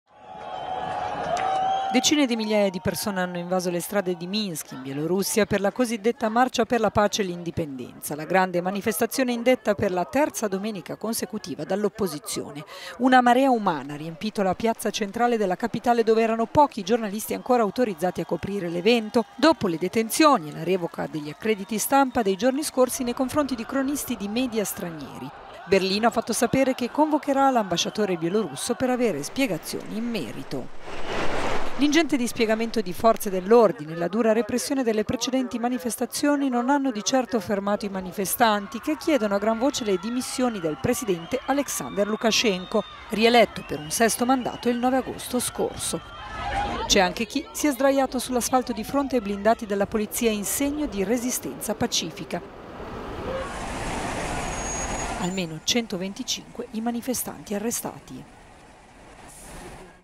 Decine di migliaia in piazza a Minsk contro Lukashenko e la repressione. 125 gli arresti
Berlino e Parigi chiedono spiegazioni sul ritiro degli accrediti stampa ai giornalisti stranieri